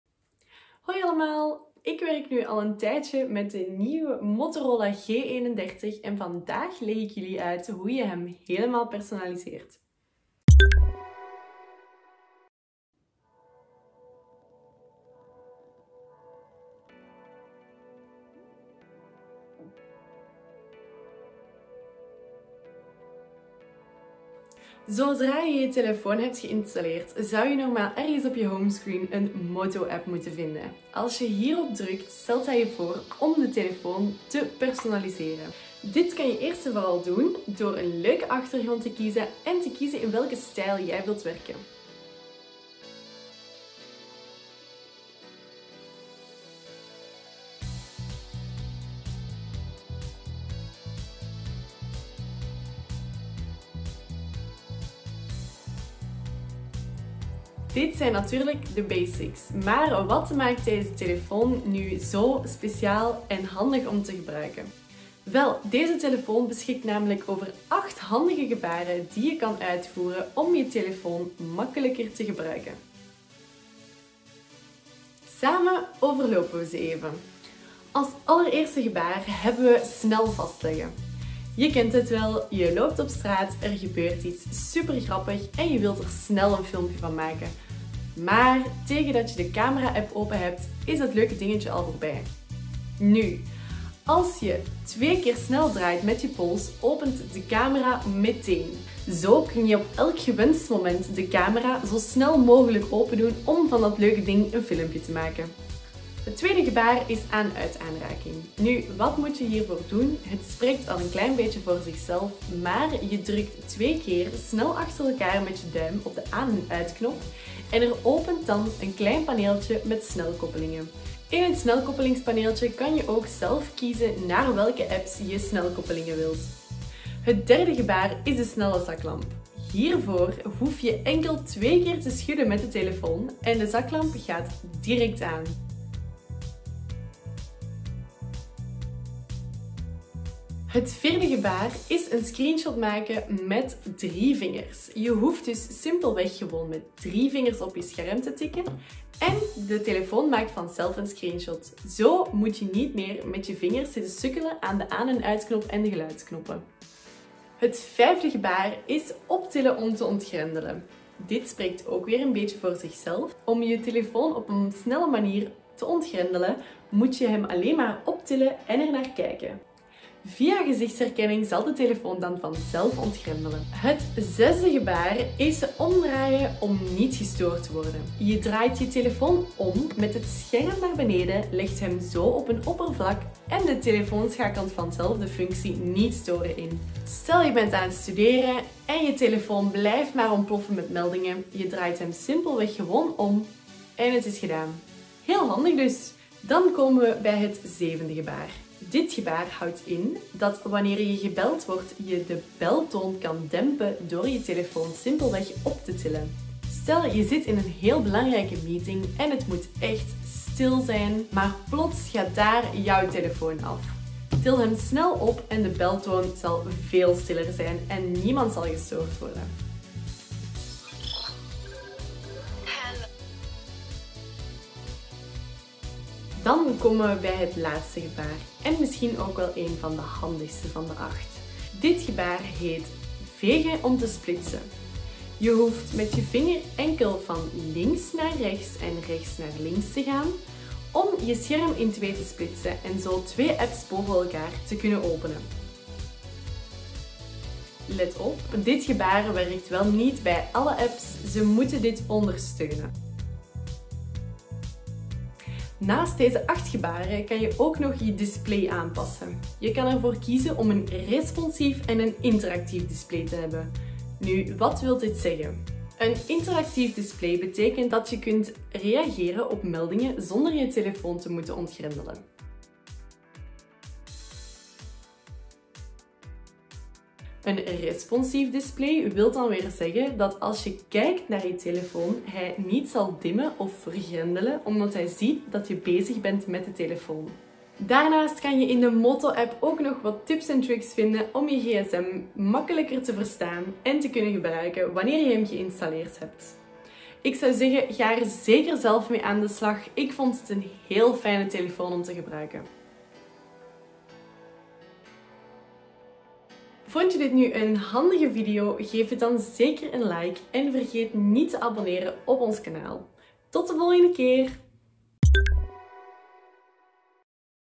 8 Slimme Moto Acties op Motorola-telefoons die je moet kennen!
We tonen je de handige Moto-acties. Dat zijn slimme gebaren op Motorola-telefoons waarmee je handige acties kan uitvoeren. Denk aan tweemaal schudden om de zaklamp te gebruiken of je telefoon draaien om de camera te openen. We leggen uit hoe het werkt.

00:00 - Intro
01:16 - Snel vastleggen
01:45 - Aan/uit-aanraking
02:06 - Snelle zaklamp
02:19 - Screenshot met 3 vingers
02:36 - Optillen om te ontgrendelen
02:53 - Omdraaien voor Niet storen
03:15 - Optillen om te dempen
03:44 - Vegen om te splitsen
04:15 - Meer slimme Motorola-functies
05:04 - Outro

Androidworld is de grootste Androidcommunity van België en Nederland.